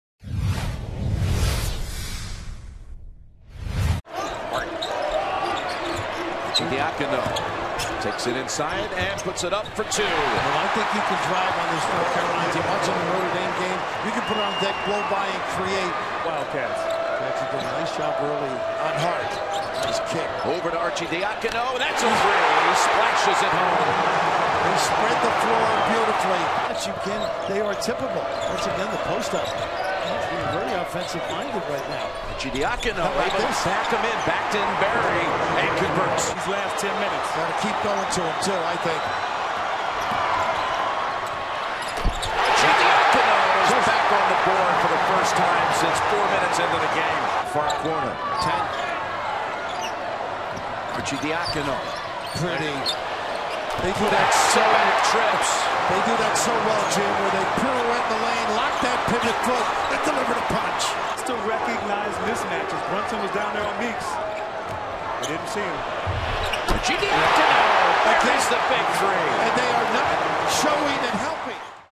Villanova's Ryan Arcidiacono goes for 16 points in win over North Carolina
Ryan Arcidiacono scores 16 as Villanova defeats North Carolina in the 2016 NCAA Tournament National Championship game.